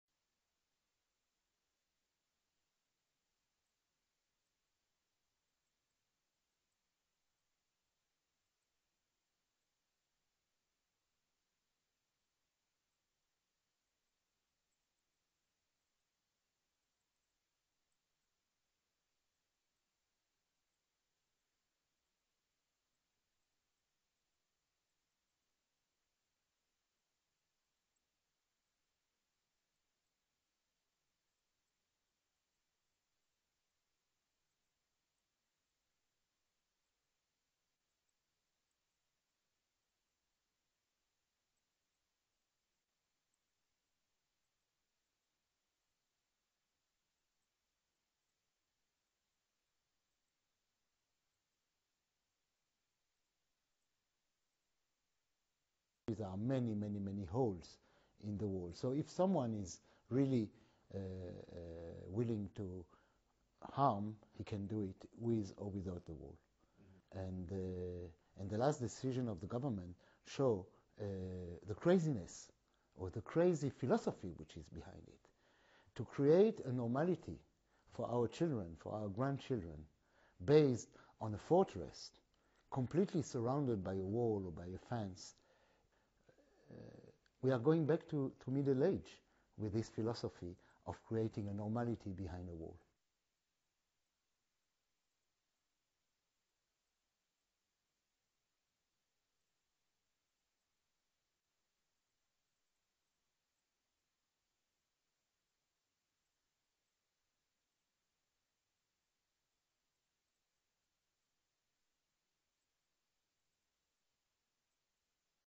Jan 30, 2013 Israel_Defensive walls increase instability in Israel
If you were allowed to invest your money into Israel's construction companies, those supplying materials for defensive walls, you would have probably made a hefty profit. Government decisions suggest they will never run out of business.

Israeli Prime Minister Benjamin Netanyahu says there is a plan to erect a fence along the boundary between the Golan Heights and Syria to defend against "infiltration and terrorism".

That would make a third defensive wall in less than 20 years - along with the one in Jerusalem and on the border with Egypt. Barriers with Lebanon and Jordan are also being discussed. All have the same justification - security reasons.

Israel built a concrete wall in the West Bank more than a decade ago to protect itself from Palestinian terrorism.

Years on, not only is it still dividing the land and the people, but opinions as well. Supporters of the defensive barrier point to a decreased number of attacks, while its opponents say its existence in the first place creates more grounds for instability.

Political writer Michael Warschawski believes the West Bank barrier may have been a temporary solution years ago, but it is no longer proving effective. Just as the much-praised fence with the Sinai Peninsula that eventually failed to keep out African migrants.

If building another turns Israel into a fortress then it will be a medieval one, Warschawski says.

"There are many-many holes in the wall, so if someone is really willing to harm - he can do it with or without the wall. And the last decision of the government shows the craziness or the crazy philosophy which is behind it -- to create a normality for our children, for our grandchildren based on a fortress, completely surrounded by a wall or by a fence. We are going back to Middle Age with this philosophy of creating normality behind a wall."

The fence at the border with Egypt can be excused for being an international border. The Golan Heights, on the other hand, is only a ceasefire line, left over from Israeli-Syrian warfare. So, it would not protect Israel from any kind of modern weapon attack and just like the wall in the West Bank it could become another monument to the indefinite occupation of territory conquered by force.